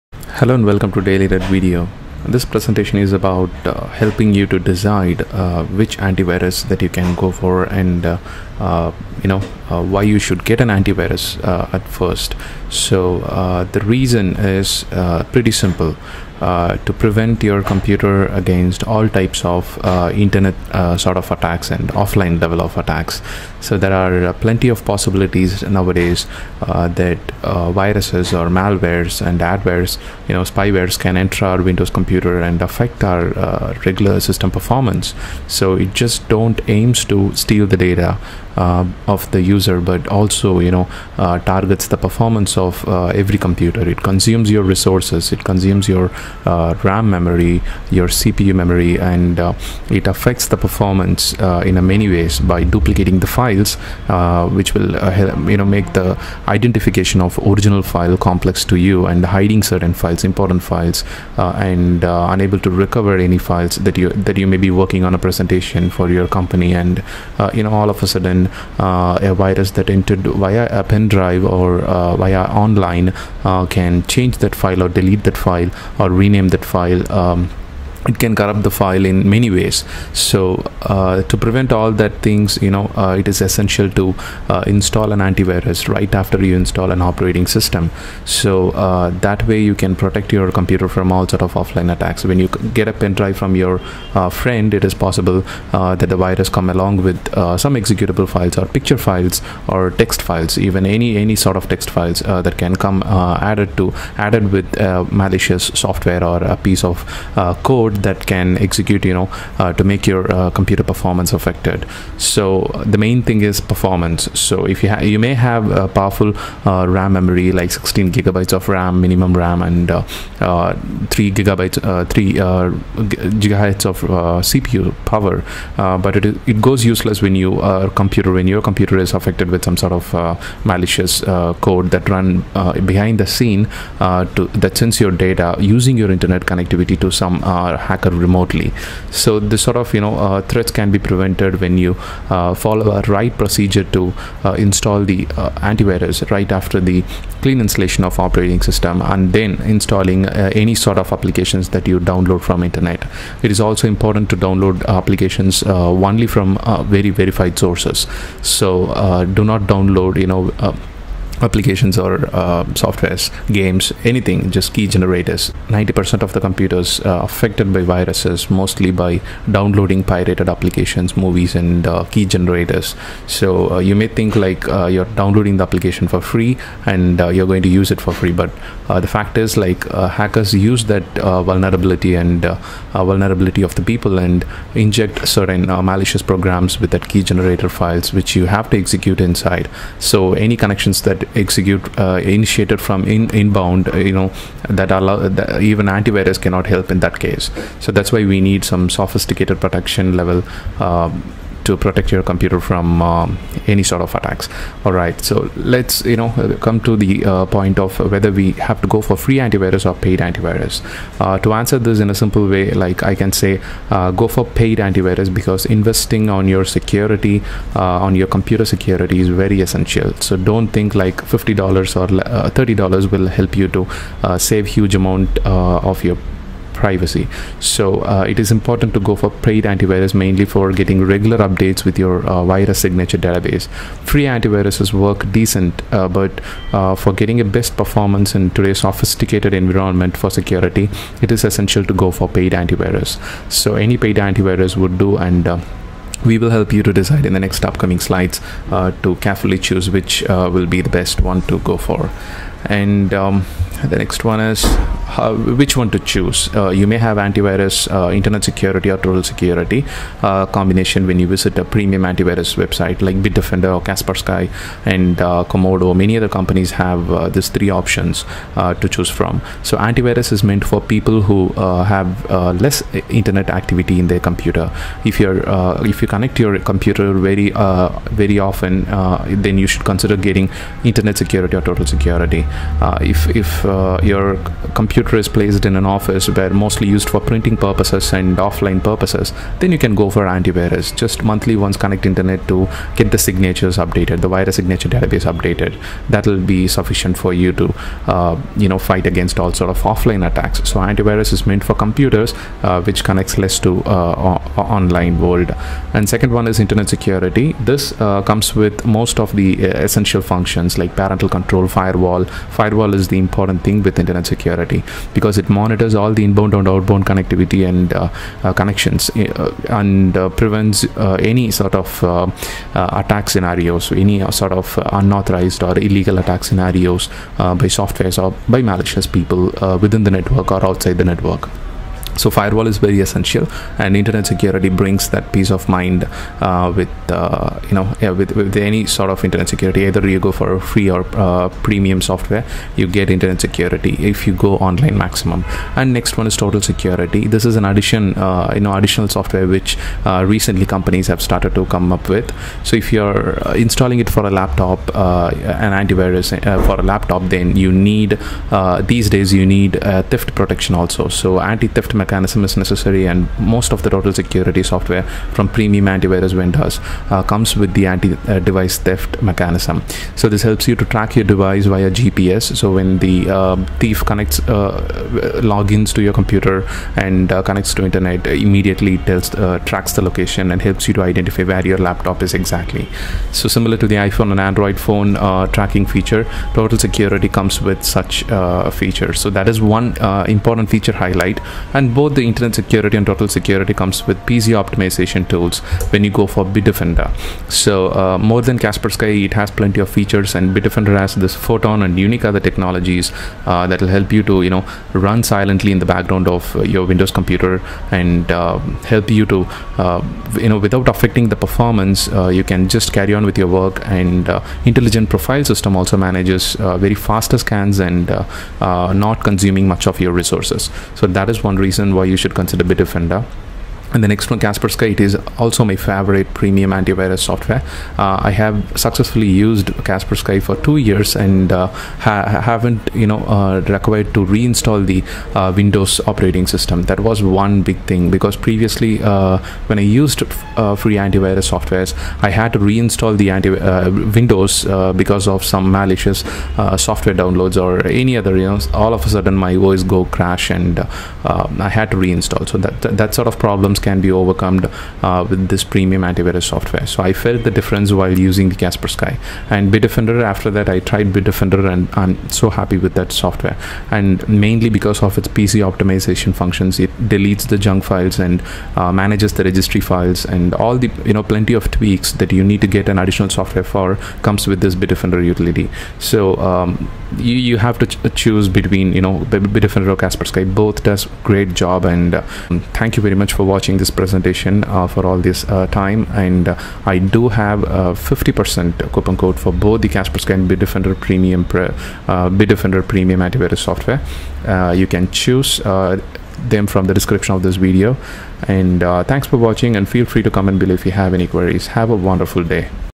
Best Antivirus Software for Windows 8/10
If you are searching for Best Antivirus software for Windows 8, then check out this video. I tried my best to recommend you a security software to protect you from all types of viruses, worms, malwares and spywares. Also compared the difference between Antivirus vs Internet Security vs Total Security Software.

Thanks for considering our recommendation and have a wonderful productive day.